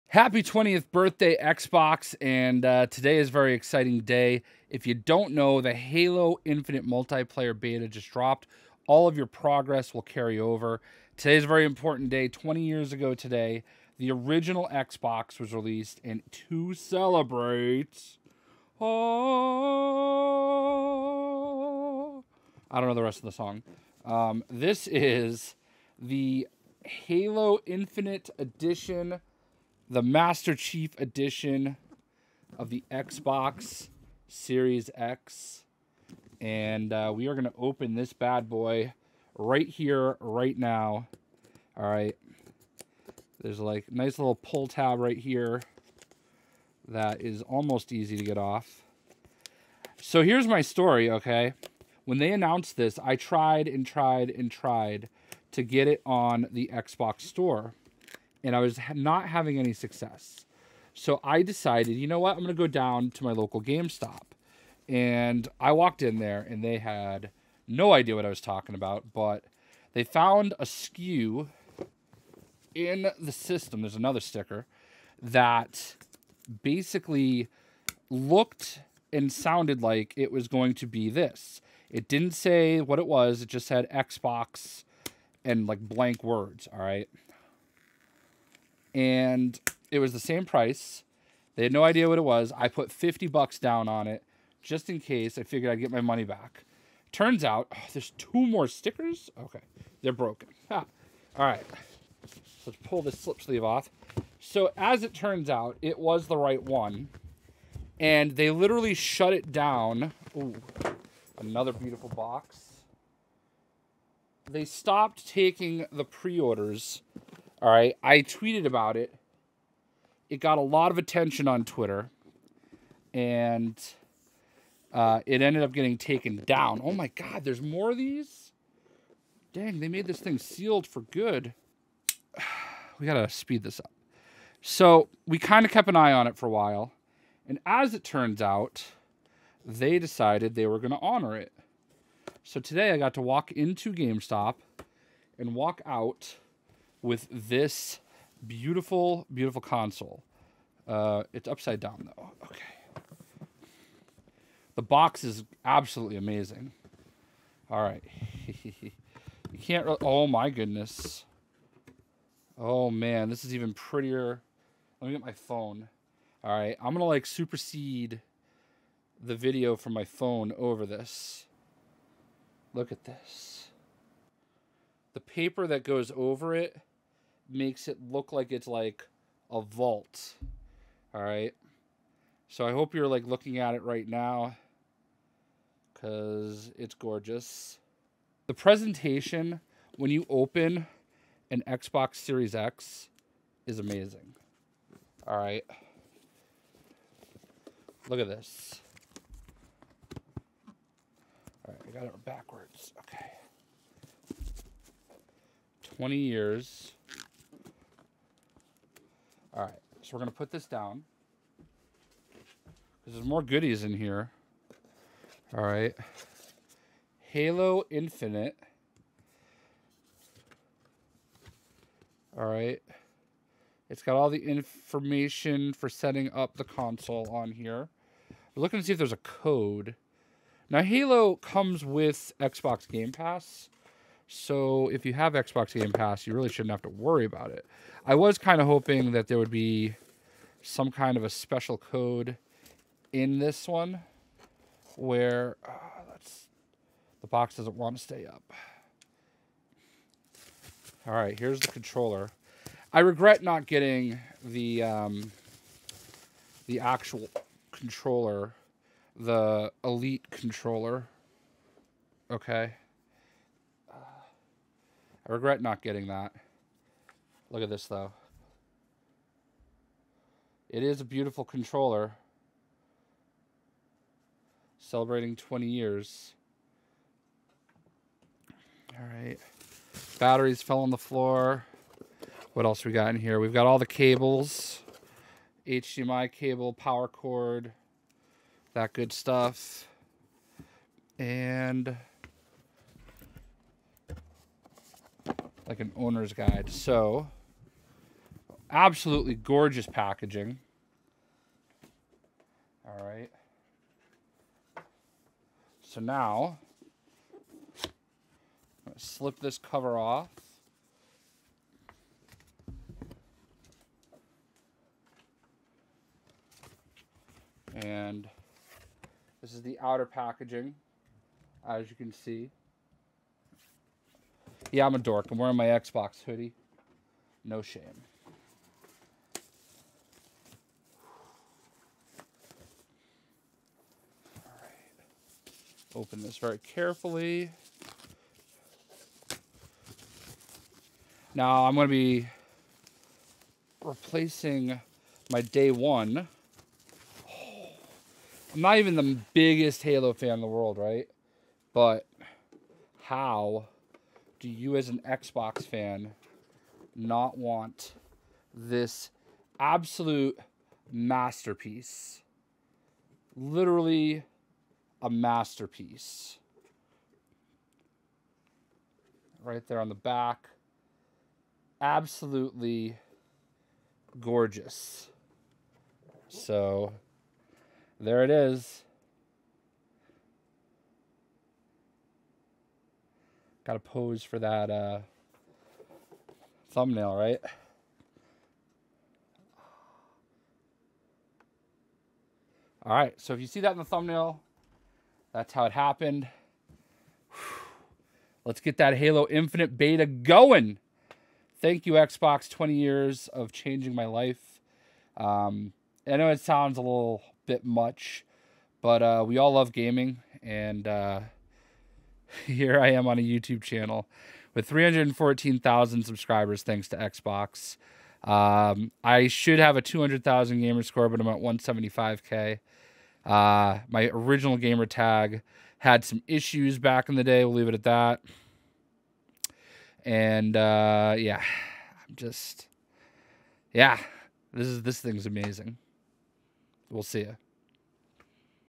Xbox Series X Halo Infinite Limited Edition Unboxing!
Today marks the 20th Anniversary of the original Xbox and Halo: Combat Evolved and to celebrate, Xbox has released it's first Limited Edition Xbox Series X in support of Halo Infinite! They have also launched the Multiplayer for Halo Infinite three weeks before the December 8th final release in the form of a beta, but all progression will carry over!

"Celebrate Halo’s 20th anniversary and confront the most ruthless foe the Master Chief has ever faced with Xbox Series X Halo Infinite Limited Edition Bundle. Includes a custom console design imprinted with stars as seen from the surface of Zeta Halo – extending onto the fan behind a blue vent inspired by Cortana."